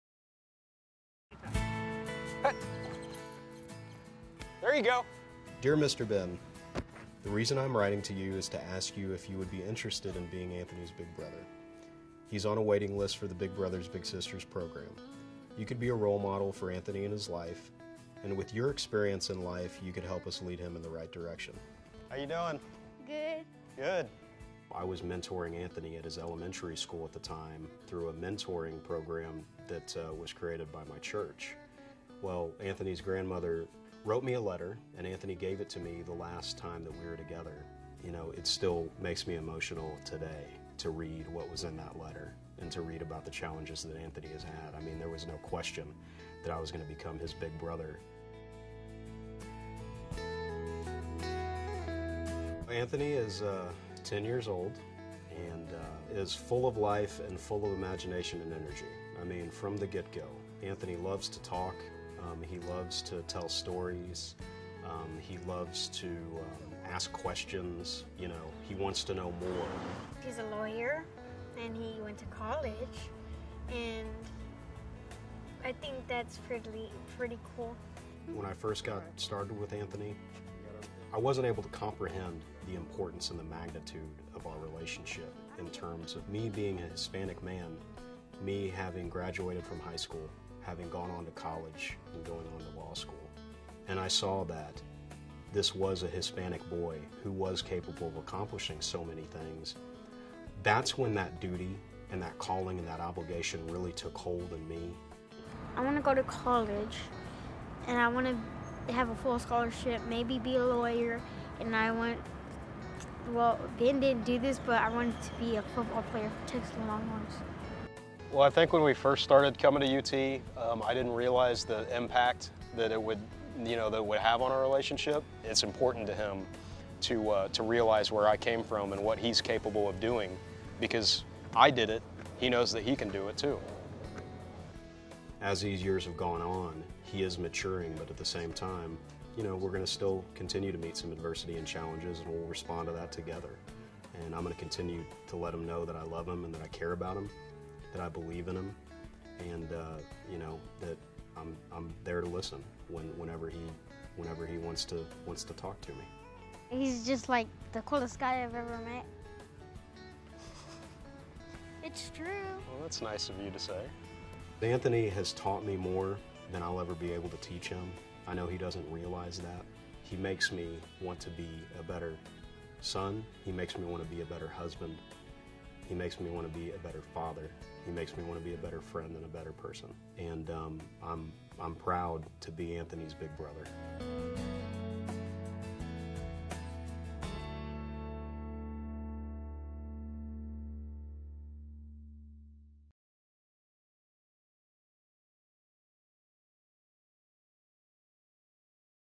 2009 Big and Little of the Year: Big Ben and Little Anthony
Ben De Leon finished high school and then went on to graduate The University of Texas at Austin with Highest Honors. Next, he attended law school and is now an associate attorney at his family-owned firm. At the time, Ben thought these were simply personal accomplishments. Little did he know that several years later, he would meet second-grader Anthony and become a role model for a young boy who wants to follow in his footsteps. While Anthony is only at the very beginning of his journey, he can see the light at the end of the tunnel thanks to Ben - 2009 National Big Brother of the Year.